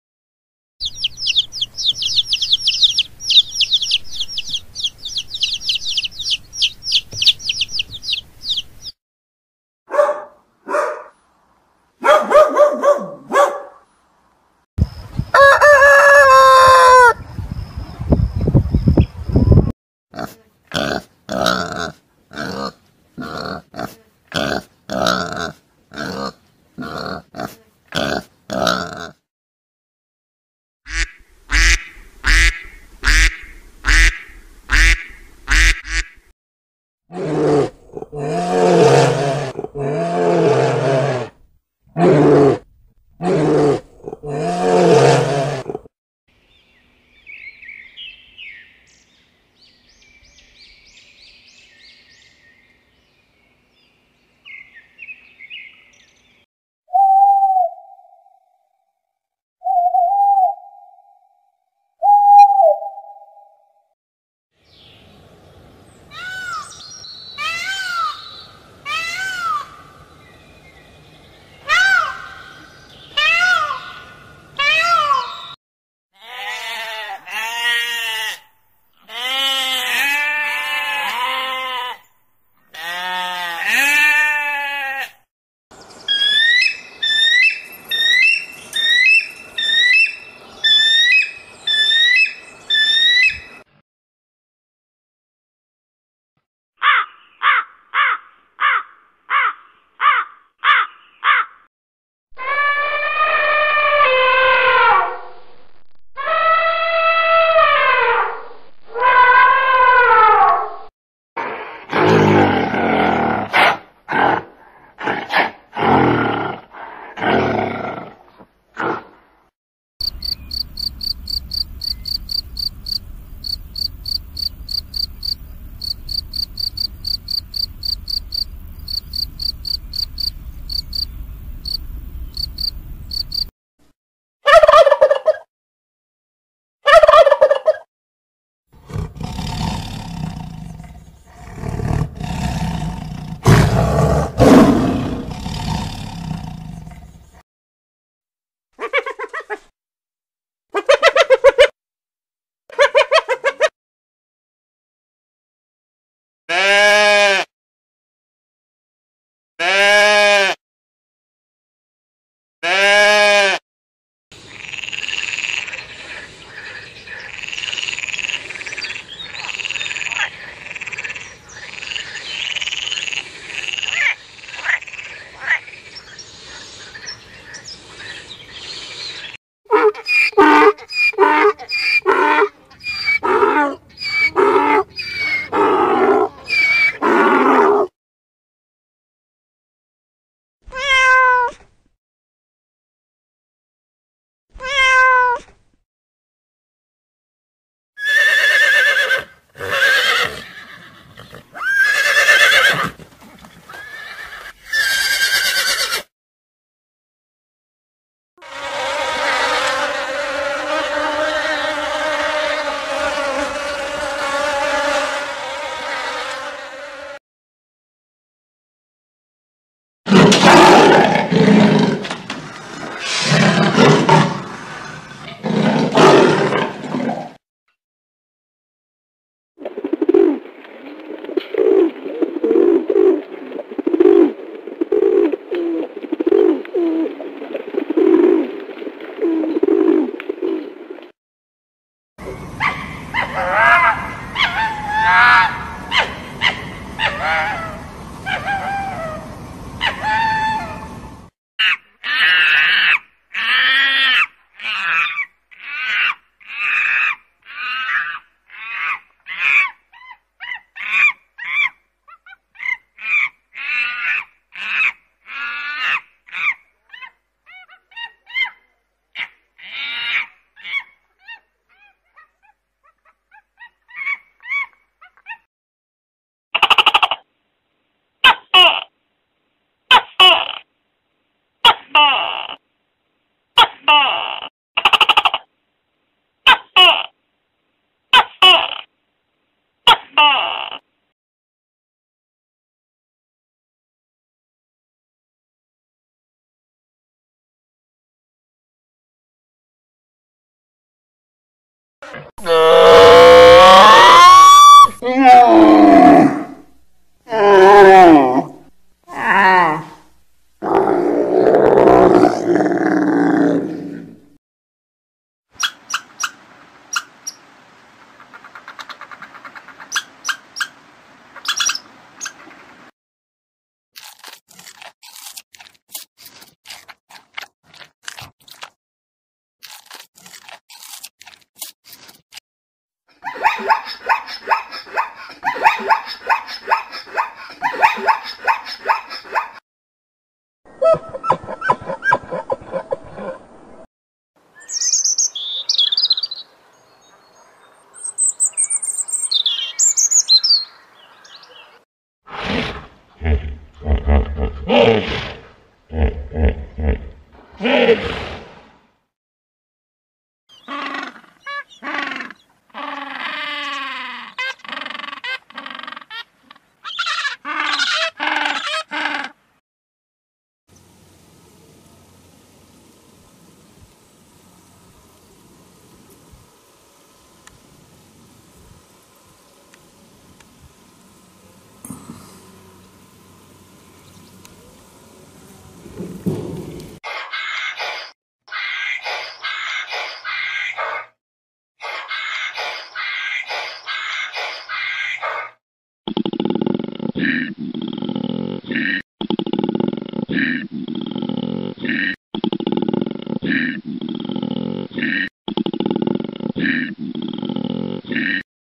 Animal Names and Sounds for Kids in English - Learn Animal Names | Animal video
Animal Names and Sounds for Kids in English - Learn Animal Names

Farm animal sounds. Farm animals for kids. Learn Farm animals.
New educational video about farm animals for children. In this video we study animals in accordance with the pedagogical methodology "Doman's Cards". We learn the words: cow, horse, goat, sheep, chicken, duck and others. We remember how they are written.